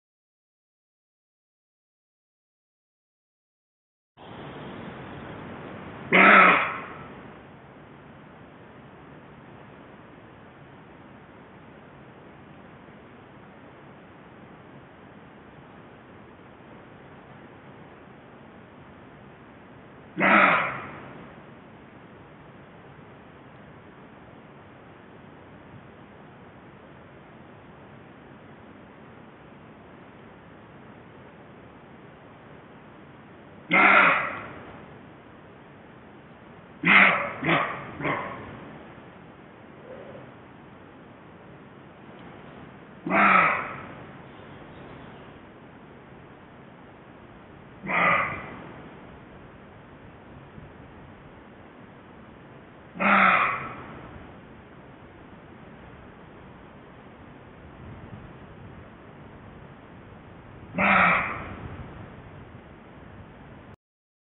E ti, quen ves sendo?
SOLUCIÓN Á ADIVIÑANZA Nº1
Efectivamente, como algunhas persoas indicaron moi ben, tratábase da ladra dun corzo macho! Os corzos emiten este son ronco e seco tan chamativo por varios motivos: como alarma, como defensa do seu territorio ante a presenza doutros machos, ou durante o celo a comezos do verán. Poden ladrar tanto femias coma machos.